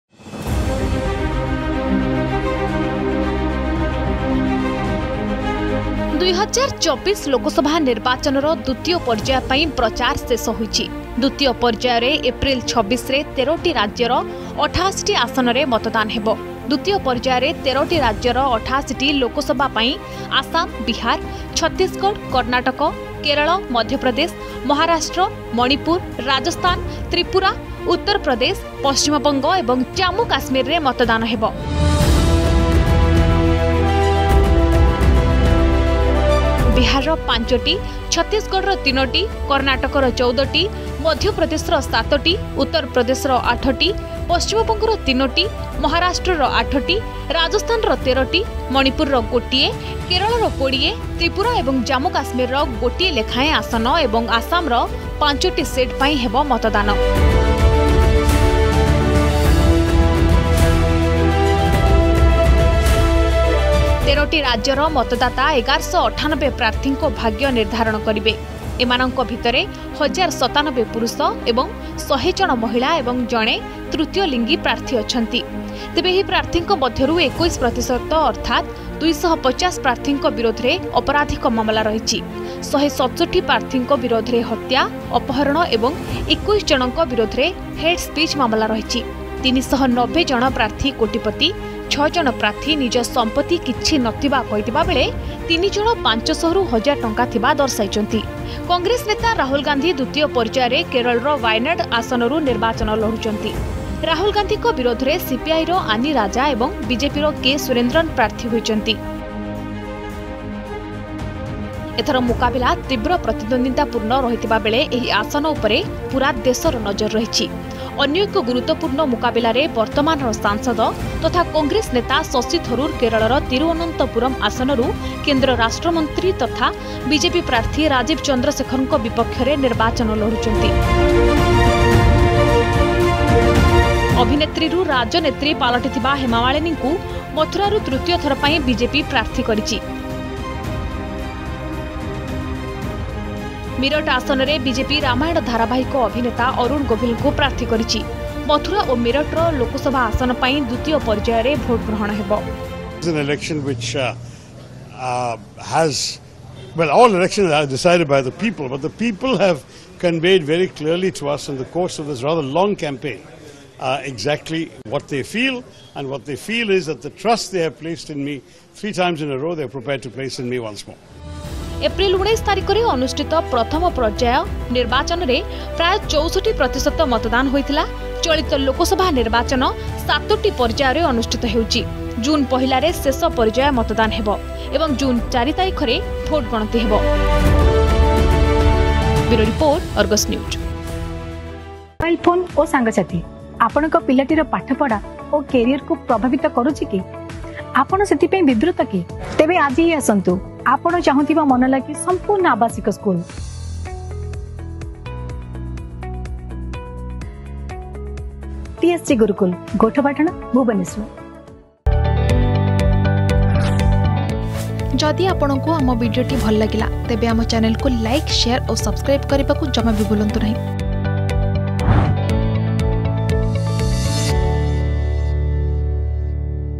୨୦୨୪ ଲୋକସଭା ନିର୍ବାଚନର ଦ୍ବିତୀୟ ପର୍ଯ୍ୟାୟ ପାଇଁ ପ୍ରଚାର ଶେଷ || Election 2024
୨୦୨୪ ଲୋକସଭା ନିର୍ବାଚନର ଦ୍ବିତୀୟ ପର୍ଯ୍ୟାୟ ପାଇଁ ପ୍ରଚାର ଶେଷ ହୋଇଛି। ଦ୍ବିତୀୟ ପର୍ଯ୍ୟାୟରେ ଏପ୍ରିଲ ୨୬ରେ ୧୩ଟି ରାଜ୍ୟର 88ଟି ଆସନରେ ମତଦାନ ହେବ।

୨୦୨୪ ଲୋକସଭା ନିର୍ବାଚନର ଦ୍ବିତୀୟ ପର୍ଯ୍ୟାୟ ପାଇଁ ପ୍ରଚାର ଶେଷ || Election 2024